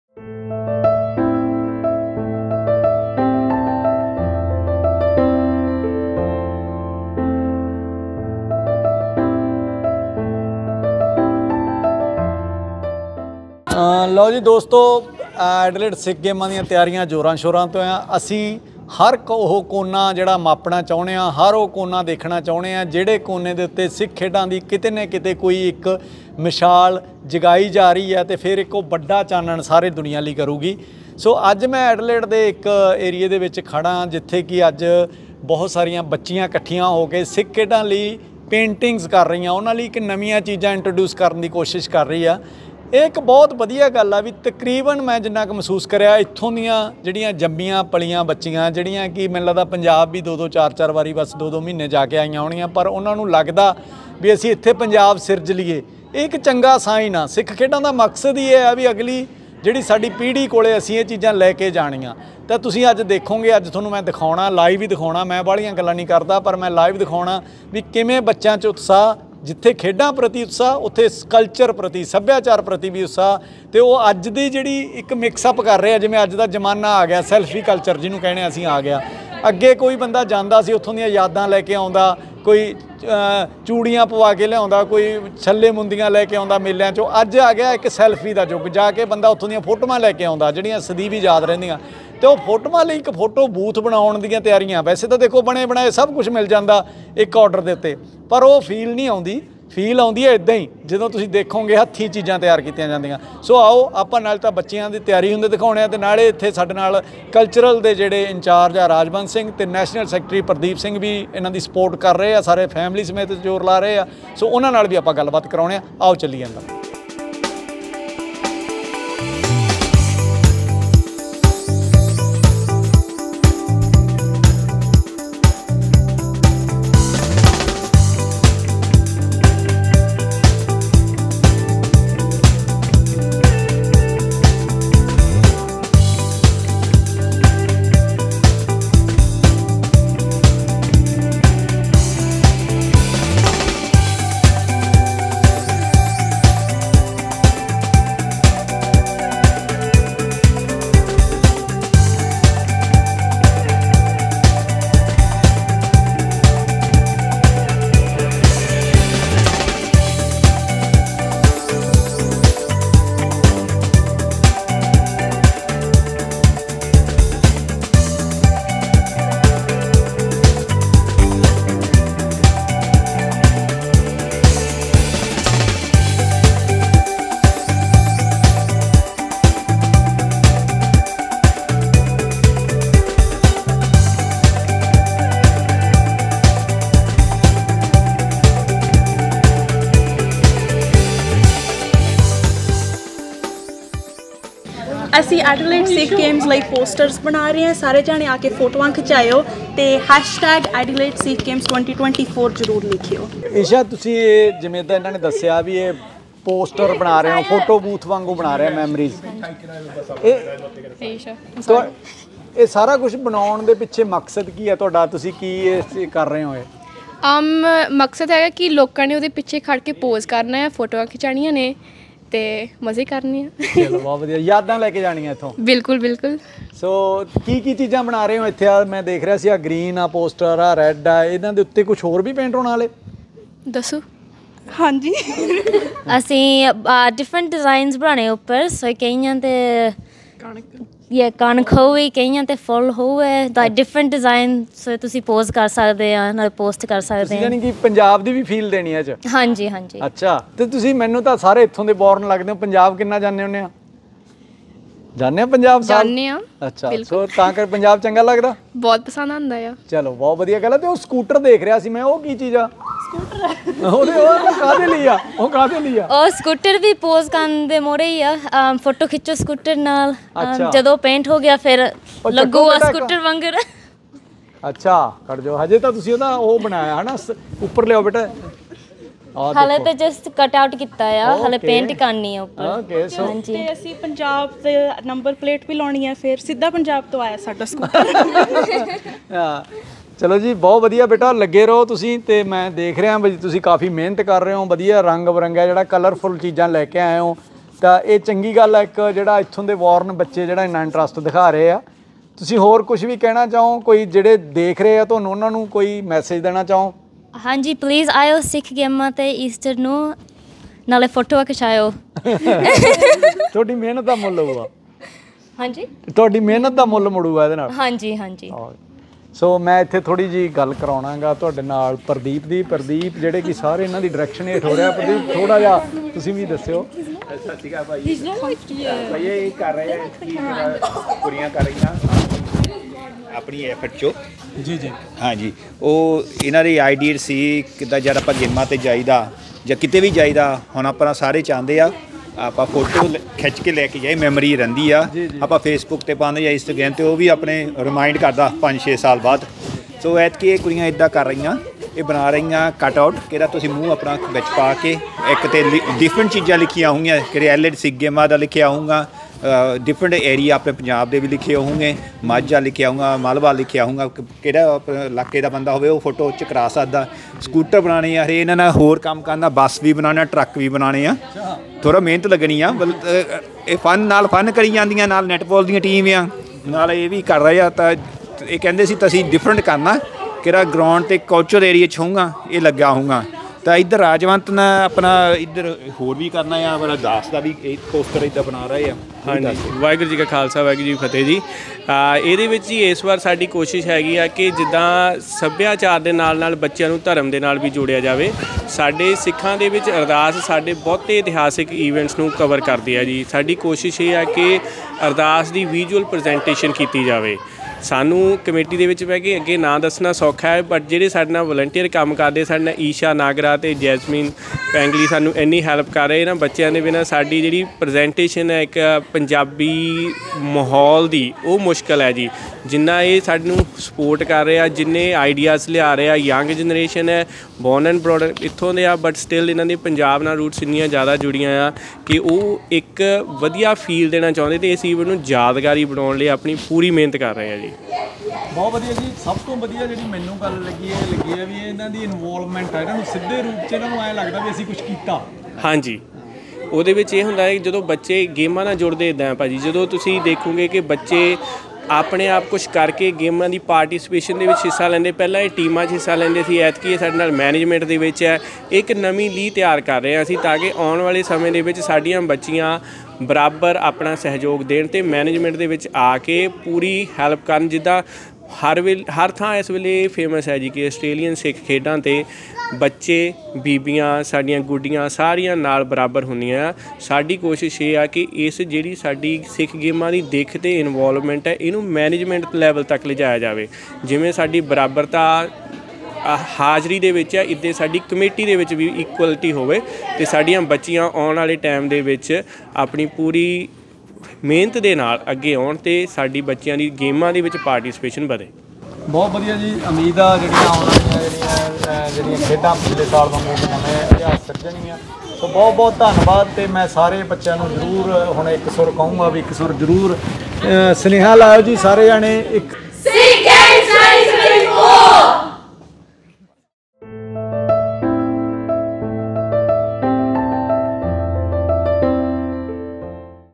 ਸਿੱਖ ਖੇਡਾਂ 'ਚ ਇਸ ਵਾਰ ਦੇਖਣ ਨੂੰ ਮਿਲੇਗਾ ਕੀ ਕੁੱਝ ਵੱਖਰਾ ?
ਦੇਖੋ ਇਹਨਾਂ ਕੁੜੀਆਂ ਵਲੋਂ ਕੀਤਾ ਜਾ ਰਿਹੈ ਨਵਾਂ ਉਪਰਾਲਾ !